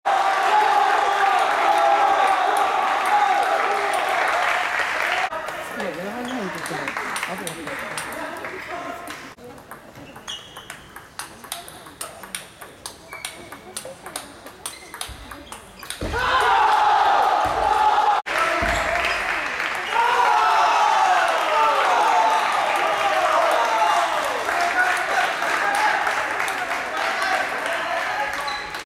シェーク・両面ペン基本打法②
2013関東学生卓球春季リーグより。財田(日大：遊学館高卒)vs定松(日体大：希望ヶ丘高卒)戦の試合開始前の練習より。シェークカットマン（裏・粒）とペン（両面裏）の基本打法です。